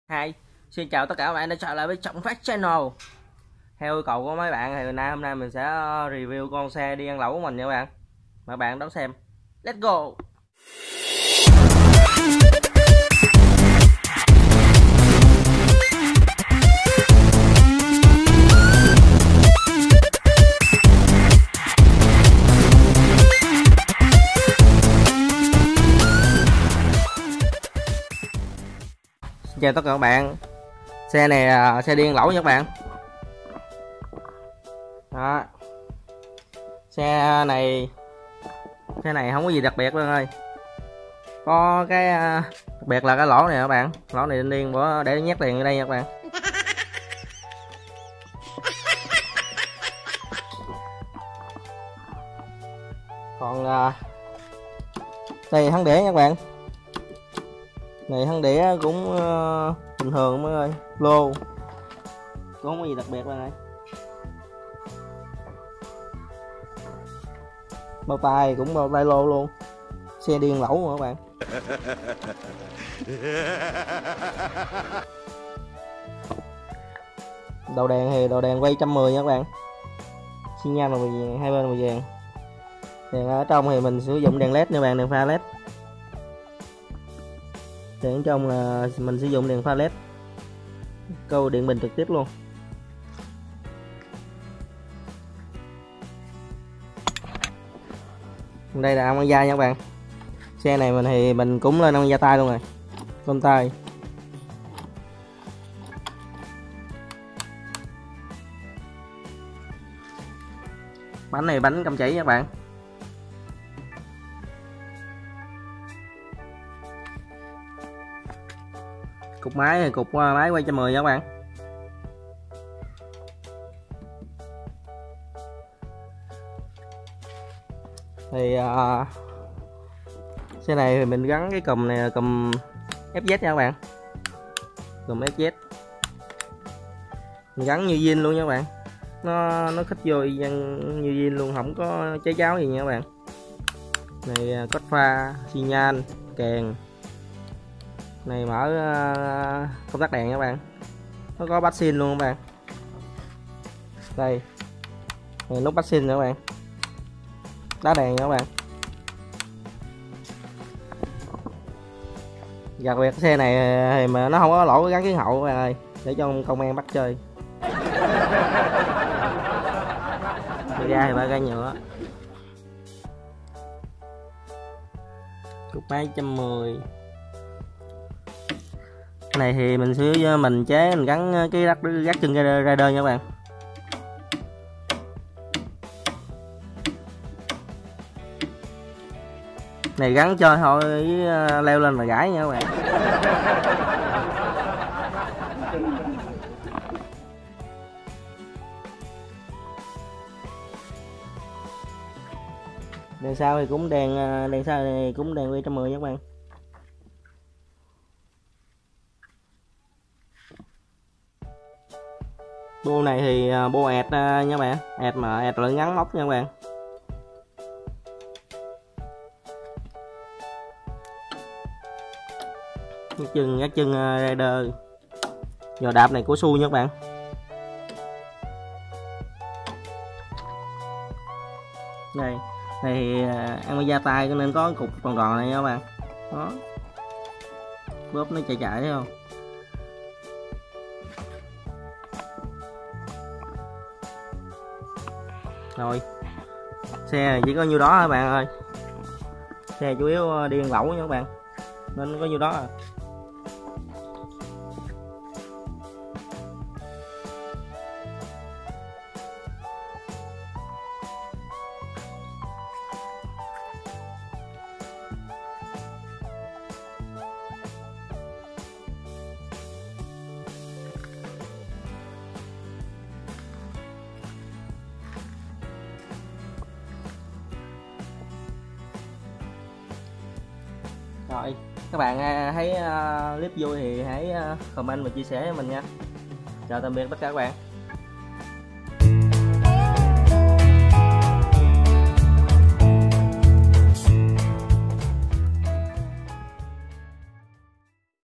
Review xe wave đi ăn lẩu | Có những gì ? —Motorbike GO eat hot pot
Xin chào tất cả mọi nguời đã trở lại với TRỌNG PHÁT CHANNEL.Hôm nai mình sẽ REVIEW CHIẾC XE WAVE ĐI ĂN LẨU của mình,mời các bạn đón xem.Lest go
Các bạn hay đăng ký ủng hộ mình nha